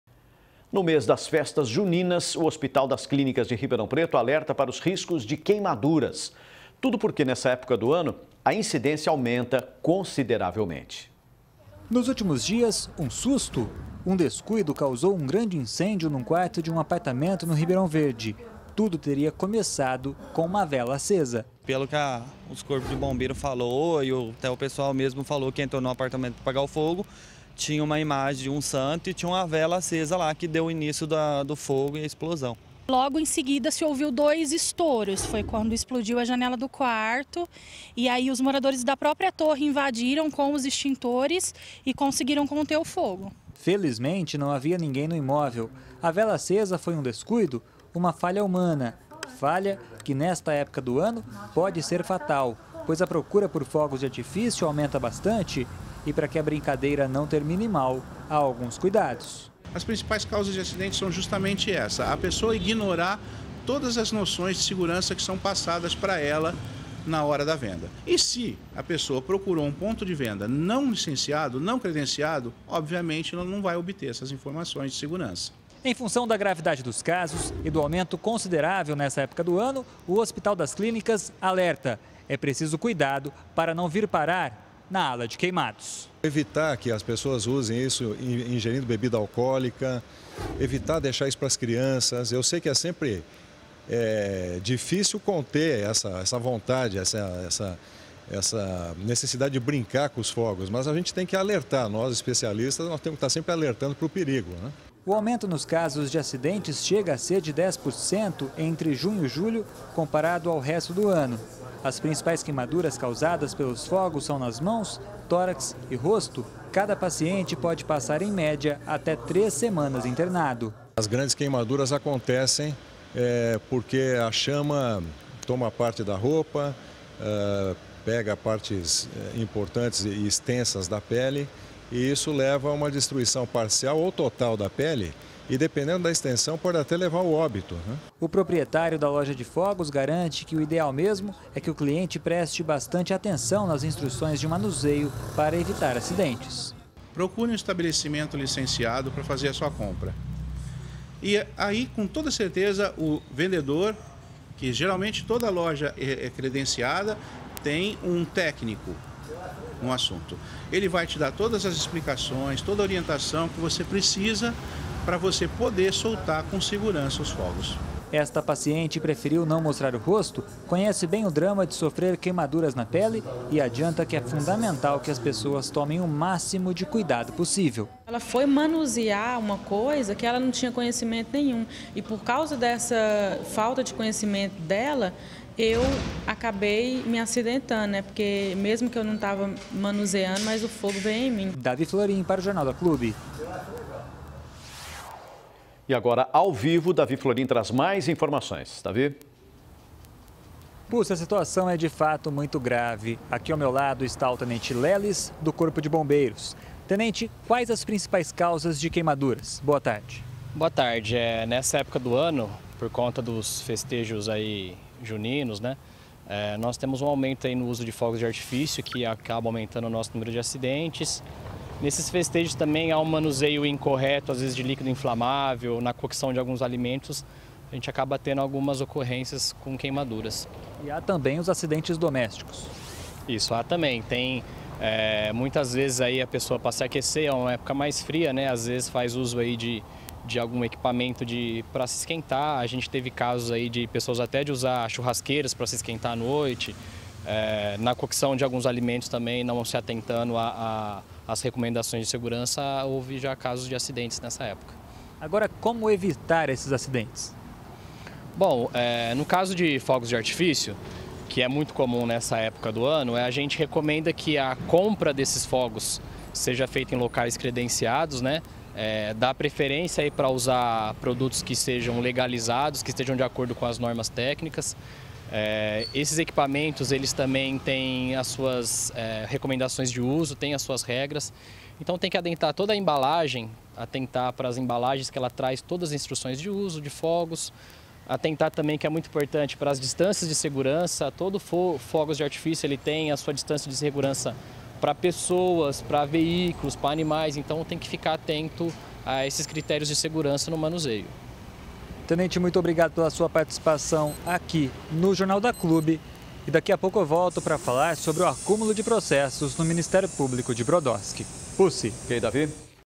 HC alerta para riscos de queimaduras no mês de festas juninas + Link - Jornal da Clube (26/06/2017)
Tudo porque nesta época do ano, a incidência aumenta consideravelmente.